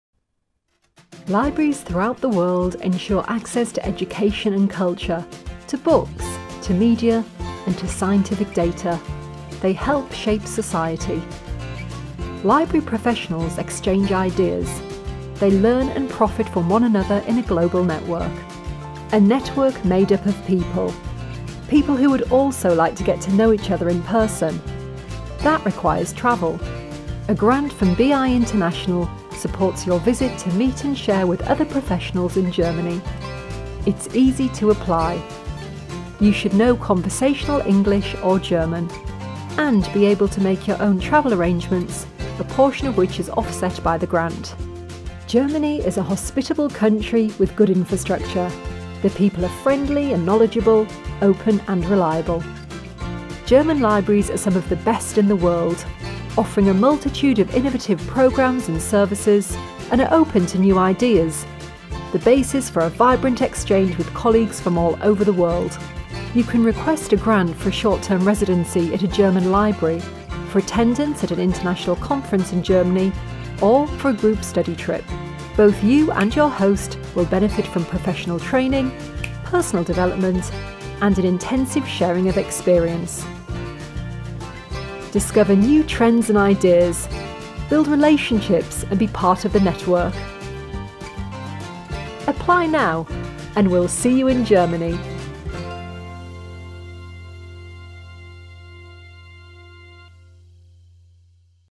BII programme AnD - visit Germany's Libraries!
Video about the grant programme AnD from Bibliothek Information International for international library professionals who want to travel for a professional stay in Germany, attend a conference there on site or virtually, or plan a group trip to Germany's Libraries.